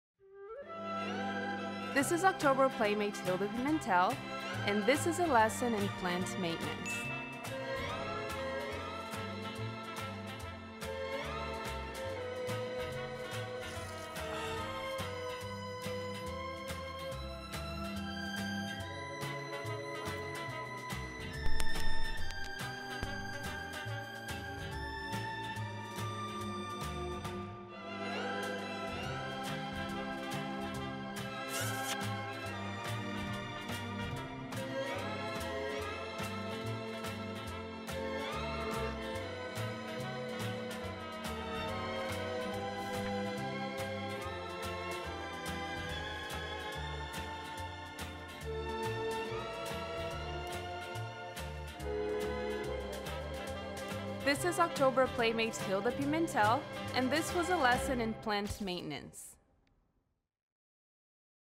A Lesson in Plant Maintenance with October 2019 Playmate Hilda Dias Pimentel
From their earliest days to the moment they leaf home, here's how to care for your house plants.